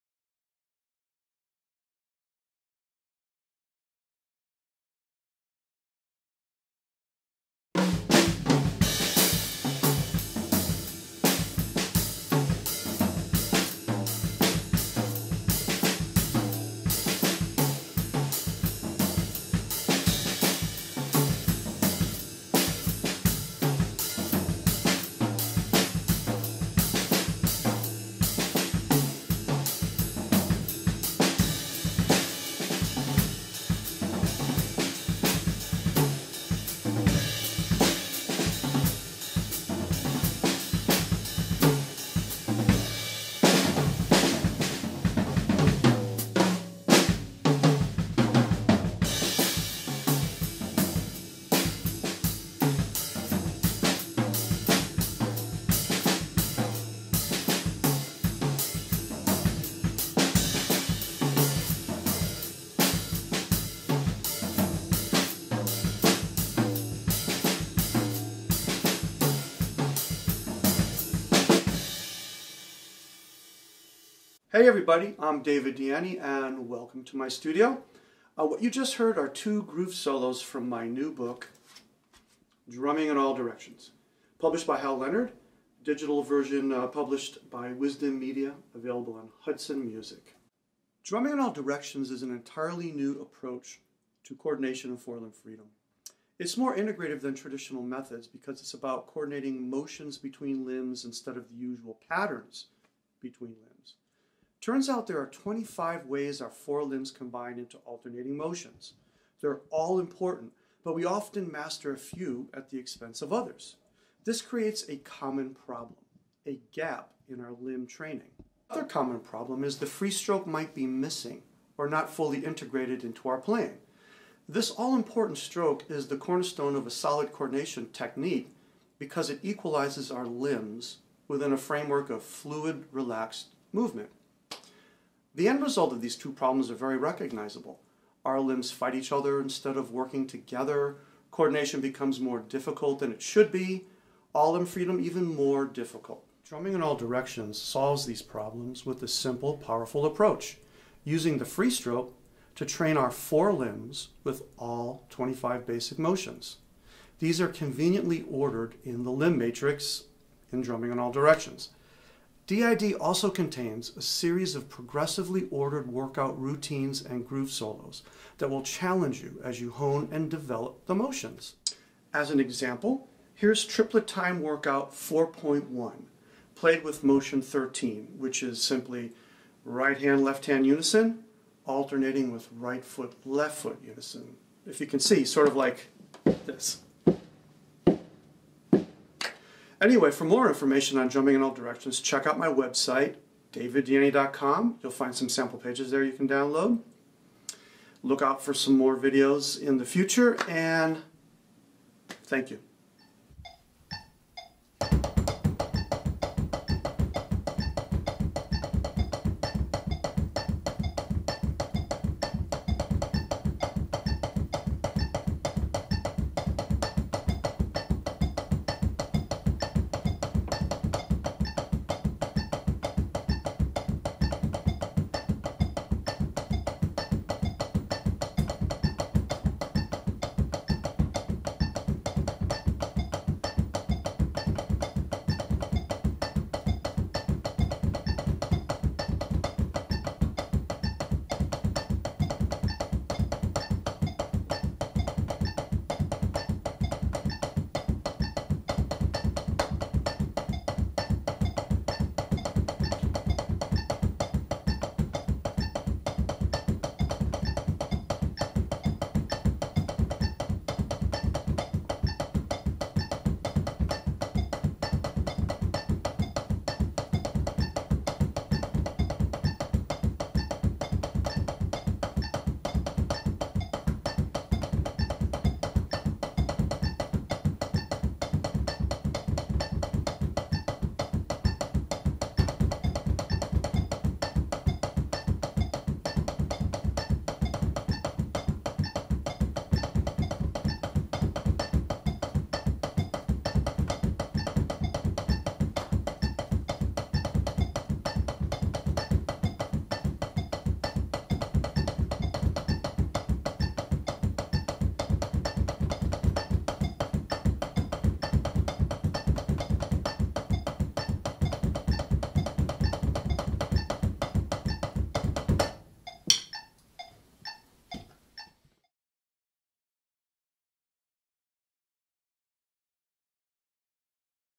Drumming in All Directions: David Dieni
A Lesson from the book Drumming in All Directions, by David Dieni. Available from HudsonMusic.com.

Frustrated with tackling coordination at the kit? Limbs tangling in the heat of battle? David Dieni's Drumming In All Directions is a whole new approach to achieving all-limb freedom and creative control. It’s for drummers of all styles who want to reach the highest level of performing!